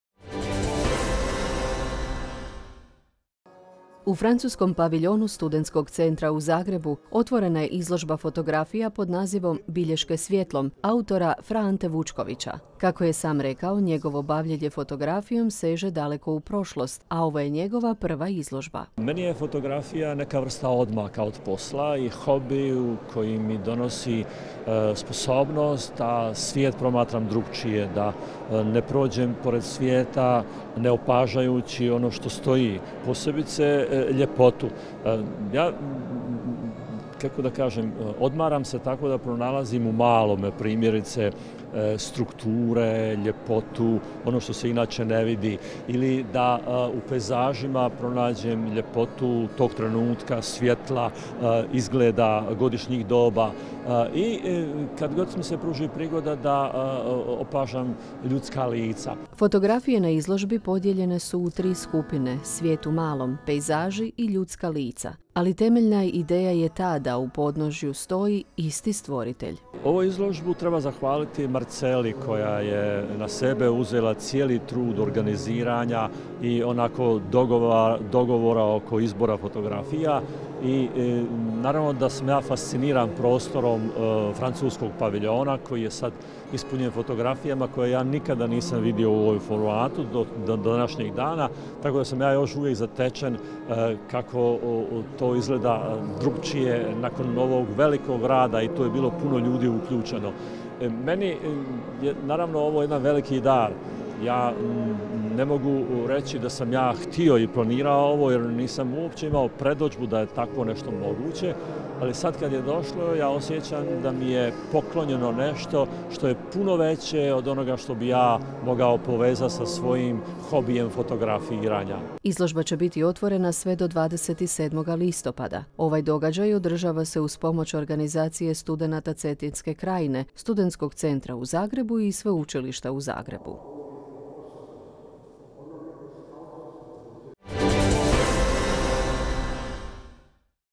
Pregled dana - Otvorena izložba fra Ante Vučkovića
U Francuskom paviljonu Studentskog centra u Zagrebu otvorena je izložba fotografija pod nazivom Bilješke svijetlom, autora fra Ante Vučkovića. Kako je i sam rekao njegovo bavljene fotografijom seže daleko u prošlost, a ovo je njegova prva izložba